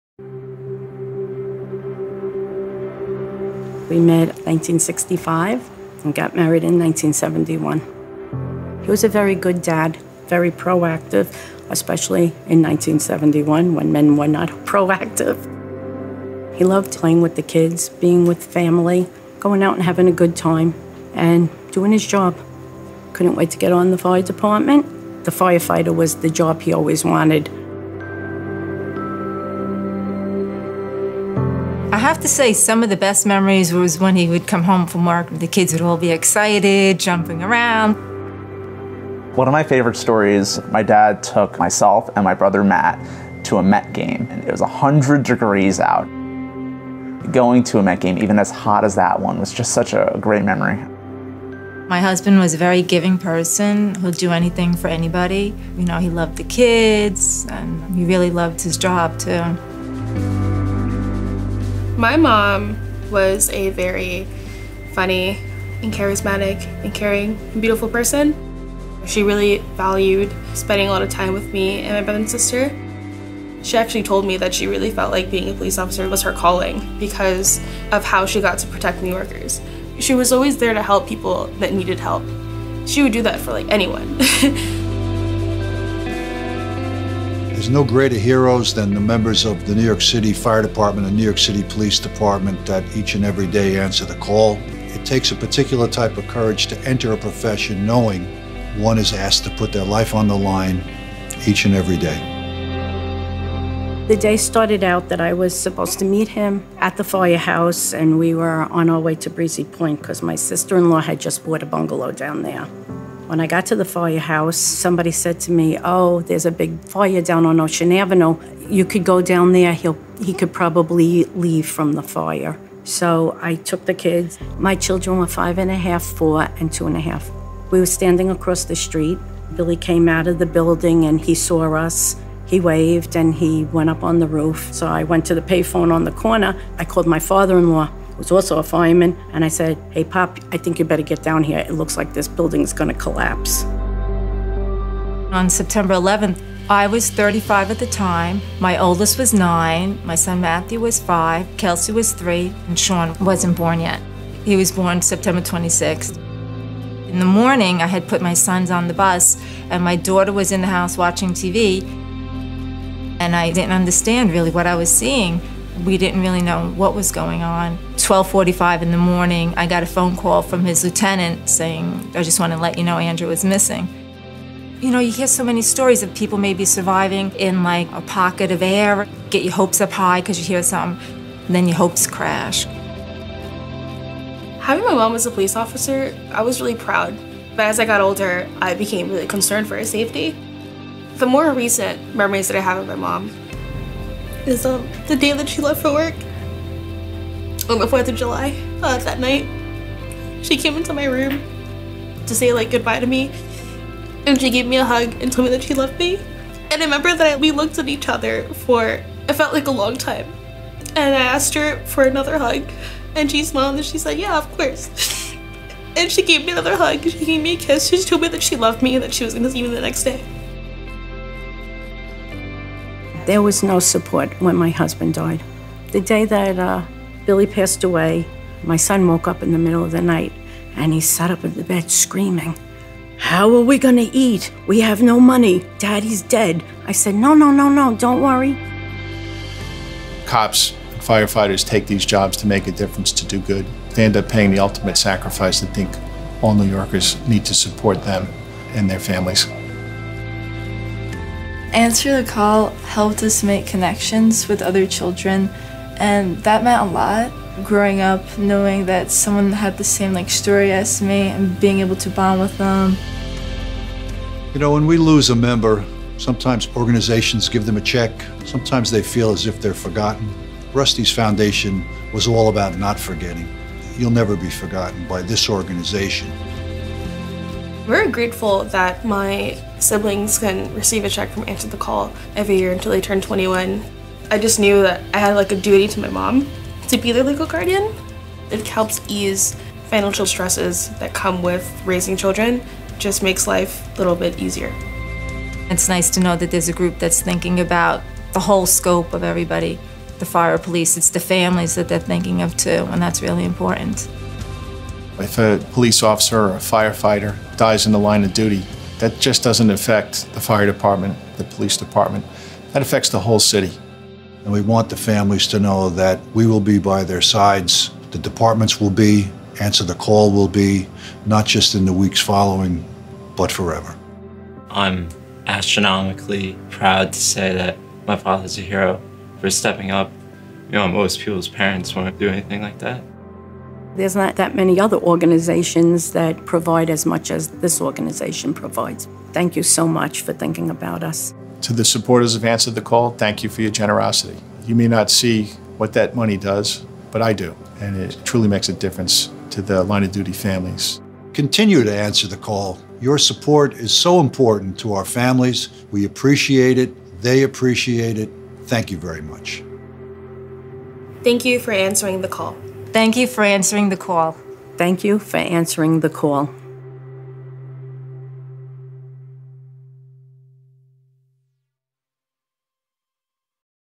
2018 Answer the Call Video
On October 10, 2018, the New York Police & Fire Widows' & Children's Benefit Fund (also known as Answer the Call) hosted its 33rd Annual Gala.  This video was played during the gala and features beneficiaries and family members of the organization: The Jordan Family, Louise O'Connor, and Genesis Villella. Also featured are NYPD Commissioner James O'Neill and FDNY Commissioner Daniel Nigro.